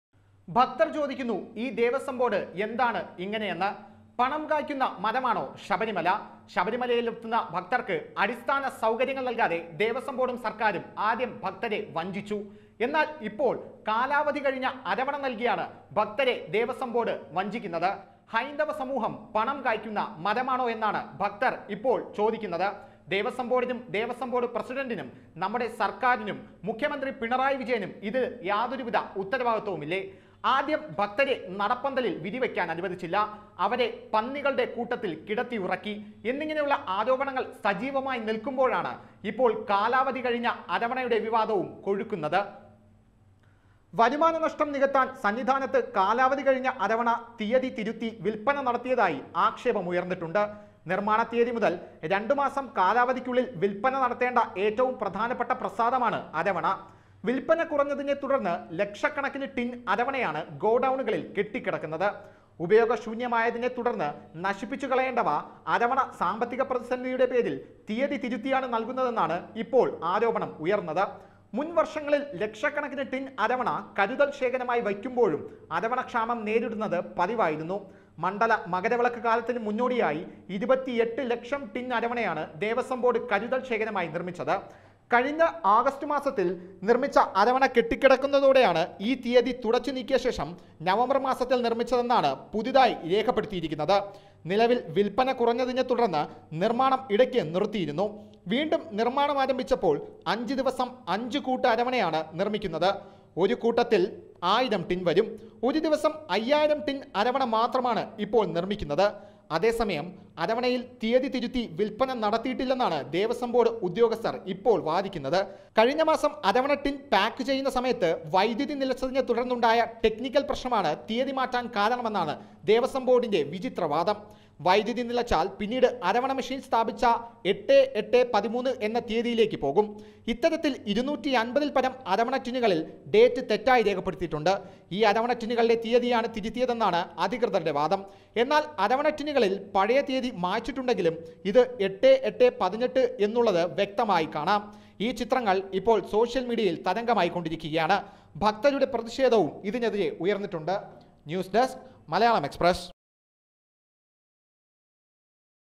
Sabarimala |ഭക്തർക്ക് കാലാവധി കഴിഞ്ഞ അരവണ നൽകി ദേവസ്വംബോർഡ് എന്ന് ആരോപണം
ഭക്തർക്ക് കാലാവധി കഴിഞ്ഞ അരവണ നൽകി ദേവസ്വംബോർഡ് എന്ന് ആരോപണം. ഇതിന് എതിരെ ശക്തമായ പ്രതിഷേധമാണ് ദേവസ്വം ബോർഡിനെതിരെ നാടെങ്ങും ഉയർന്നുവരുന്നത്. ശബരിമലയിൽ ഭക്തരോട് ദേവസ്വംബോർഡിന് യാതൊരു കൂറും ഇല്ലെന്നും ഭക്തരുടെ കുറവും ഇല്ലെന്നും ഭക്തരുടെ കാണിക്കയിലാണ് ദേവസ്വം ബോർഡിൻറെ കണ്ണെന്നും ഇതിലൂടെ തെളിഞ്ഞിരിക്കുകയാണ് എന്നാണ് സോഷ്യൽ മീഡിയയിൽ അടക്കം ഉയരുന്ന പ്രധാന ആരോപണം